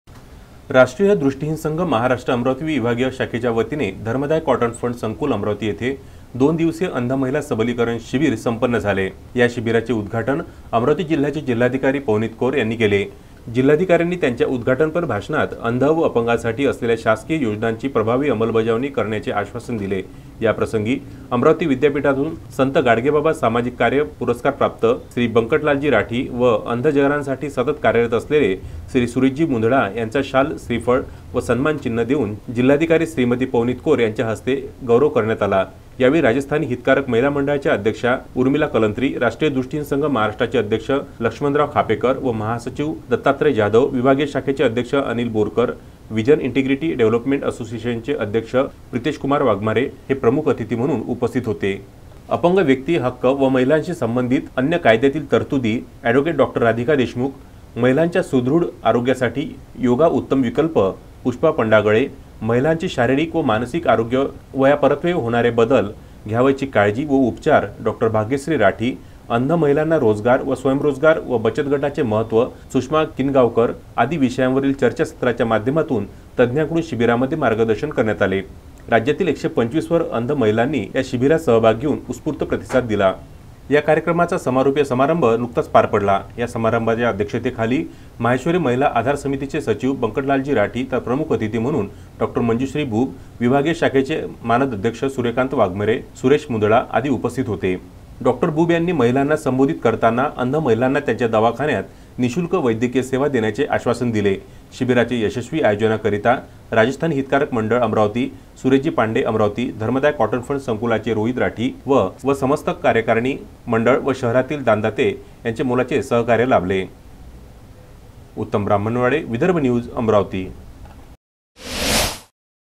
Amravati | अंध महिला सबलीकरण शिबिर संपन्न
दिव्यांगासाठी शासकीय योजनांची प्रभावी अंमलबजावणी करणार
जिल्हाधिकारी पवनीत कौर यांचे आश्वासन